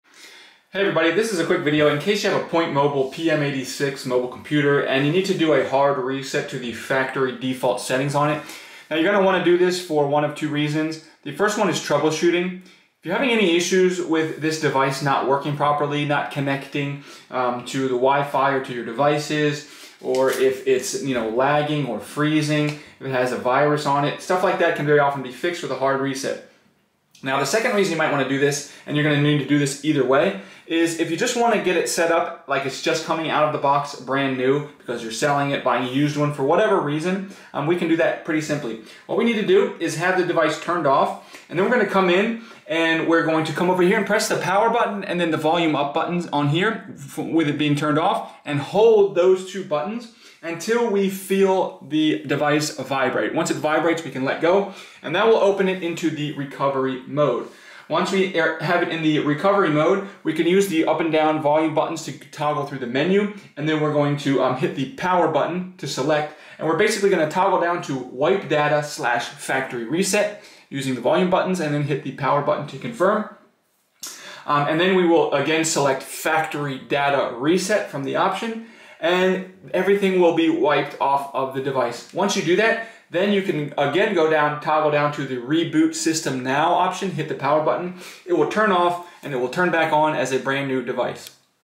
Hard Reset Pointmobile PM86 Mobile Computer
How to troubleshoot or setup starting with a hard reset a Pointmobile PM86 Mobile Computer to factory default settings if you forgot the password or if it is not working or not connecting or pairing or if it is frozen or not responding, the factory reset is a good troubleshooting step. You can also restore the factory settings to setup your device as new. Found in the Pointmobile PM86 Mobile Computer user manual.